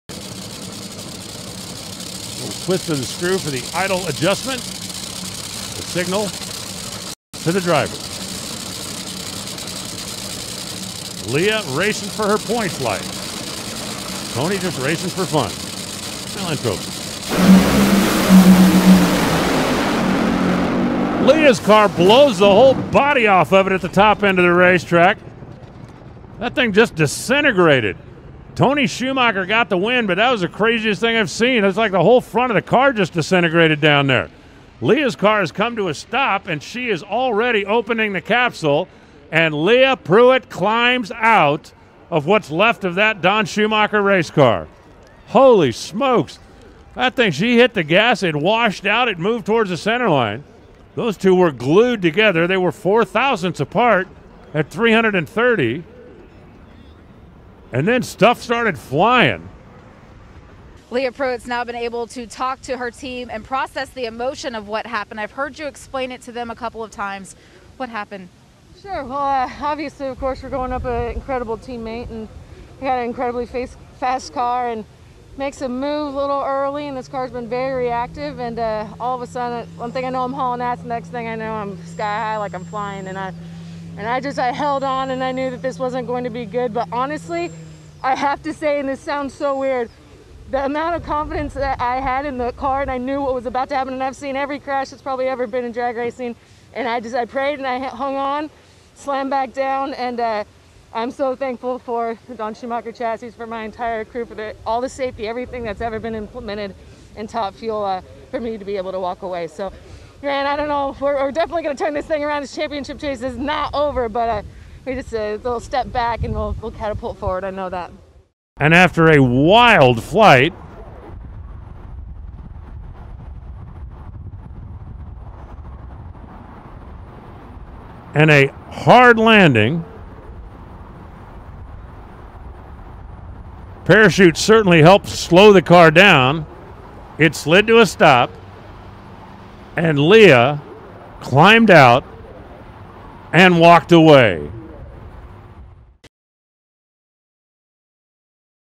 LEAH PRUETT TAKES A WILD RIDE IN ST. LOUIS
Leah Pruett can honestly say she understands the term, "and they walked away."

After a ride where her dragster went airborne and finished in pieces, she was able to do just that. She walked away.

For Pruett, the ride was eerily similar to the one experienced by Larry Dixon in March 2015. Her dragster also appeared to break apart just in front of the foot box, flexing the chassis upward where it snapped in two and sent the driver's compartment flying.

"This car's been very reactive and all of a sudden, one thing I know I'm hauling ass, next thing I know I'm sky like I'm flying and I held on and I knew that this wasn't going to be good," Pruett explained after the incident.

The accident could have been much worse had the impact of landing not been absorbed by the slicks.

"I have to say, and this sounds so weird, the amount of confidence that I had in the car and I knew what was about to happen and I've seen every crash that's probably ever been in drag racing," Pruett said. "I prayed and I hung on, slammed back down. I'm so thankful for the Don Schumacher chassis, for my entire crew, for all the safety, everything that's ever been implemented in Top Fuel for me to be able to walk away."

A defiant Pruett says the crash won't hold her back.

"We're definitely going to turn this thing around, this championship chase is not over, but just a little step back and we'll catapult forward, I know that," Pruett surmised.